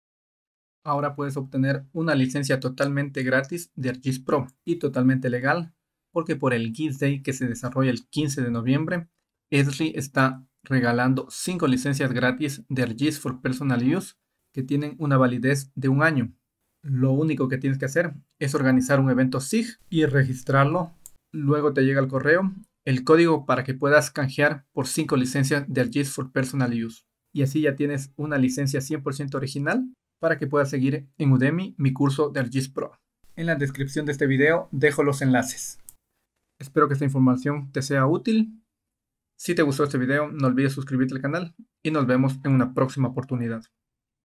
ArcGIS Pro 100% Gratis y legal
Por el GISDay que se desarrollará el 15 de noviembre si organizas un evento SIG, Esri te regala 5 licencias de ArcGISPro por un año. luego descargas e instalas.